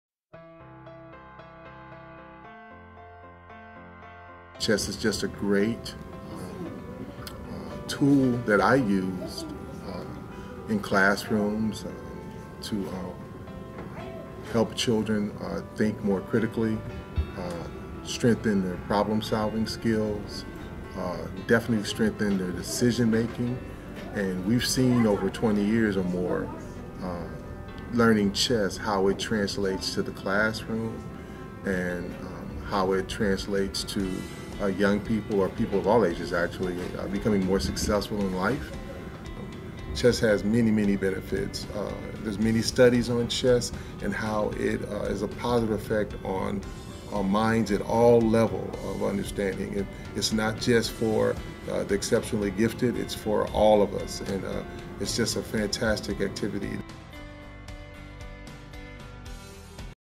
Chess Club
Learn about the benefits of chess and our chess club.